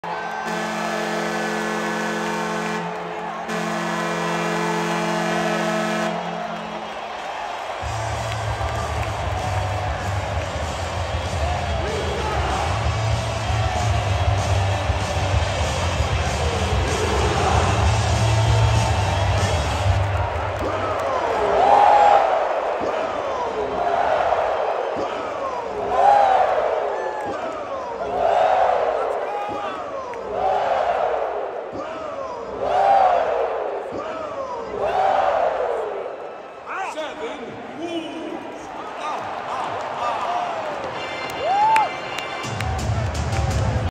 NJ Devils Jack Hughes Gives Devils 7-2 Lead vs. Carolina Hurricanes Game 3 IN ARENA CAM
Jack Hughes gives the Devils a 7-2 lead during New Jersey Devils vs. Carolina Hurricanes Game 3 Eastern Conference Semifinals

See our other videos for Devils Coverage!